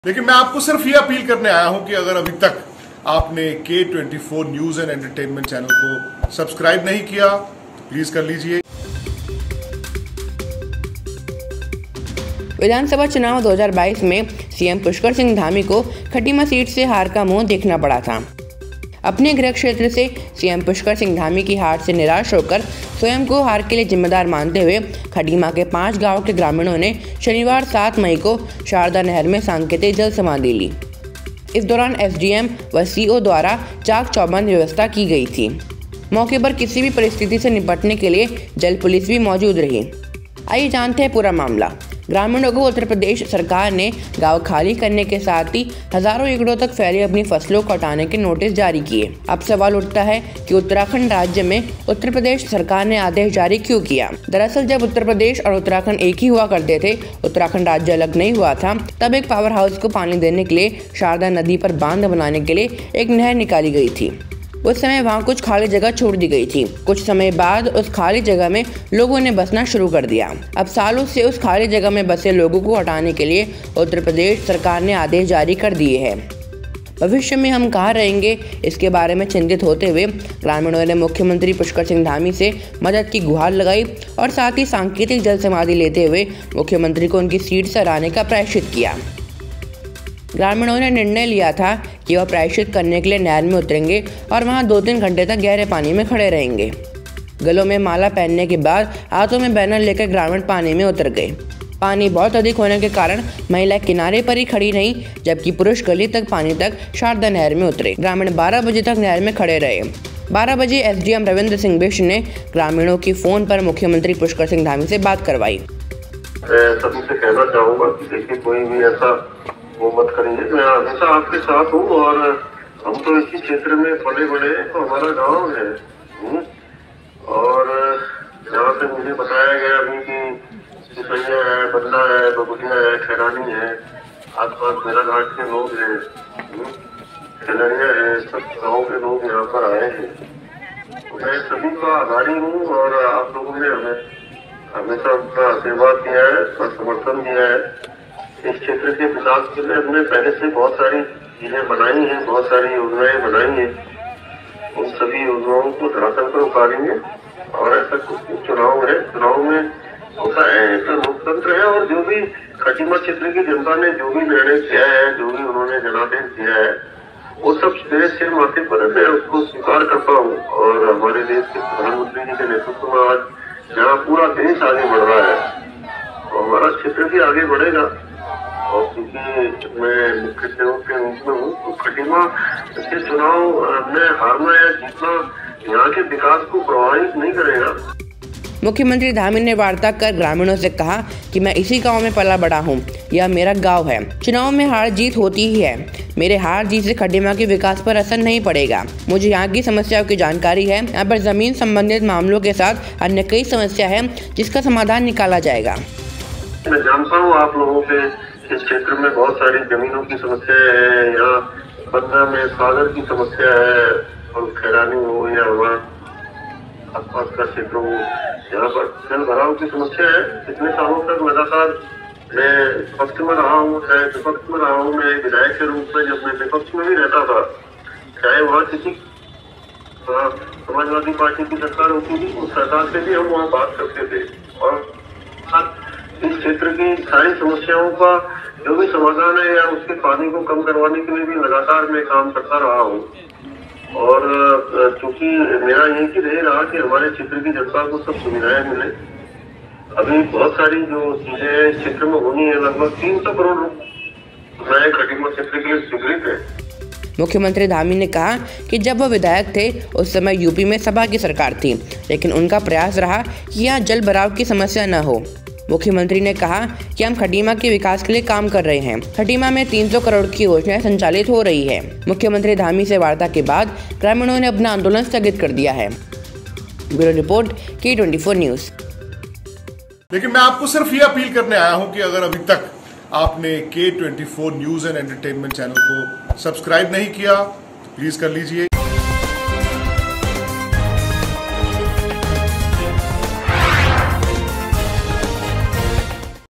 चम्पावत उपचुनाव से पहले खटीमा ने क्यों ली जलसमाधि ? II K 24 NEWS II
VIDEO BY : K 24 NEWS